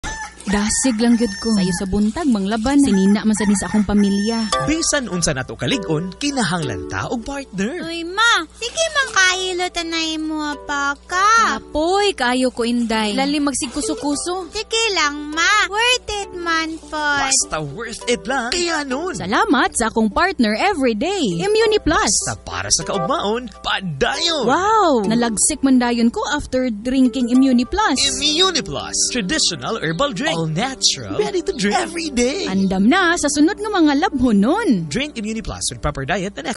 Immuni+ Traditional Herbal Drink Radio Commercial 2021 37s (Chores Version)
A Radio Commercial for Immuni+ Traditional Herbal Drink based on the Version "Chores".

DRPS Two Episode No. 1117: Immuni+ Traditional Herbal Drink Radio Commercial 2021 37s (Chores Version)